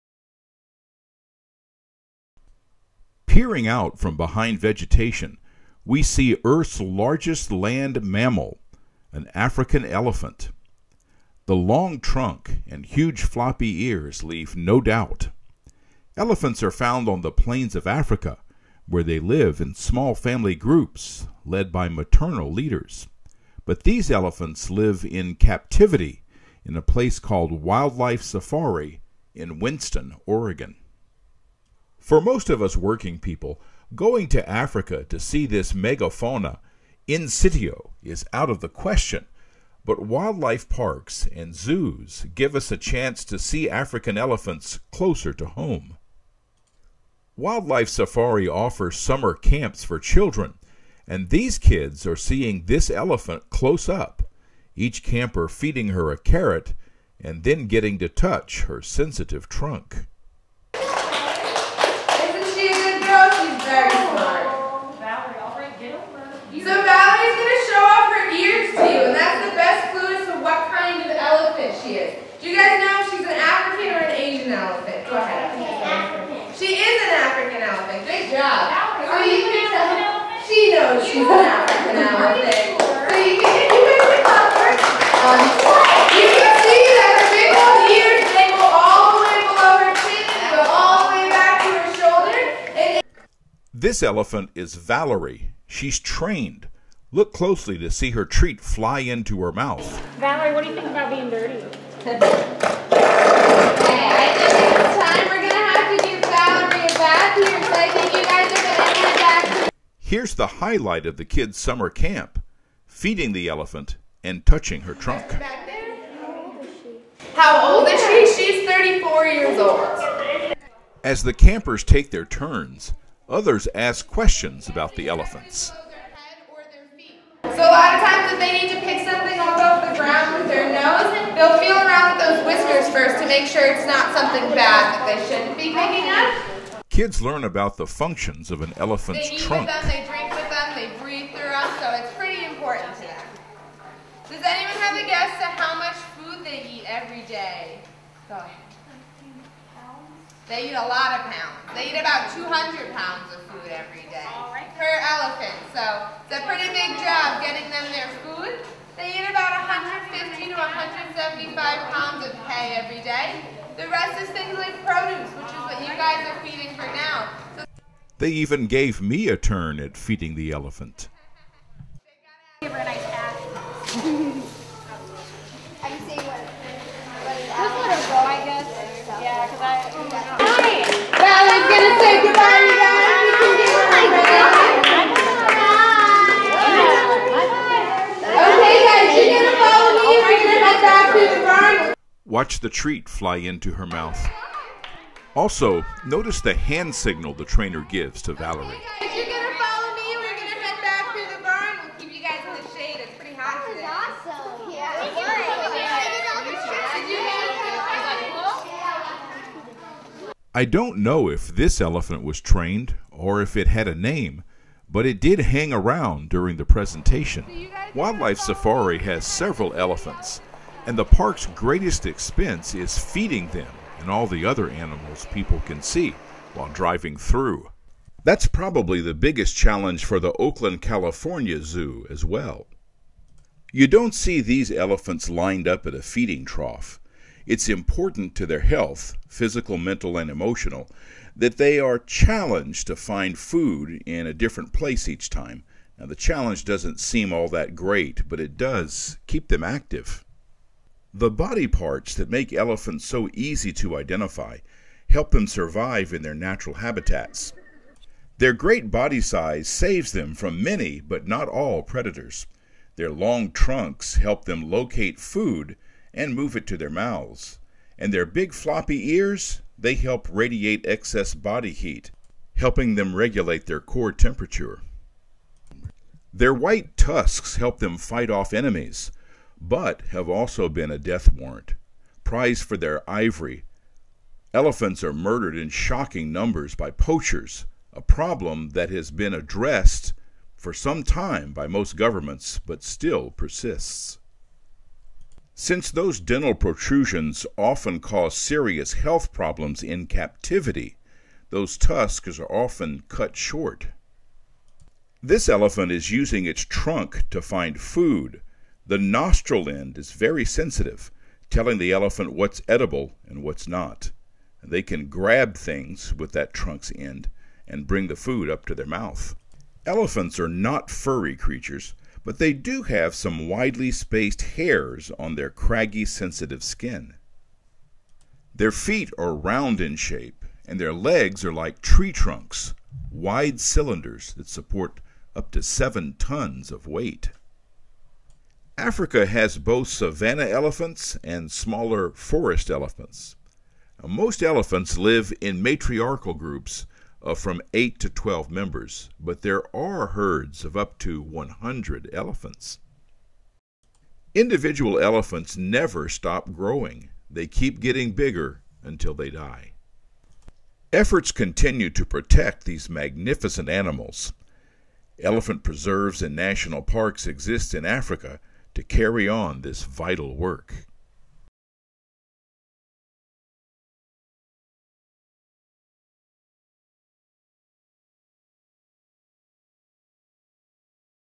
17 RUE Elephants
This video Supports Episode 56 of Ramping Up your English.  This is our first episode on African Animals. Viewers can use this episode to help them choose an animal on which to report, if the haven't already done so. Visit LetzCreate.org and go to the Episode 56 page for the entire lesson.